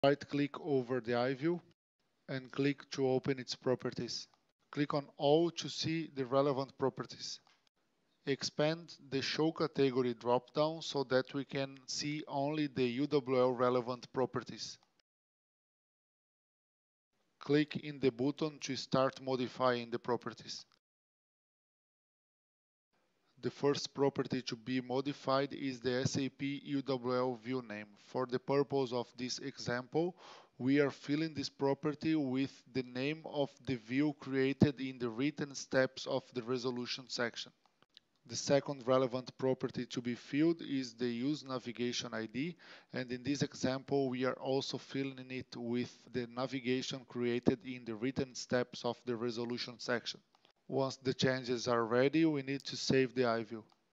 SAP ABAP: How to configure mass processing of decision tasks in the UWL2
How to configure mass processing of decision tasks in the UWL2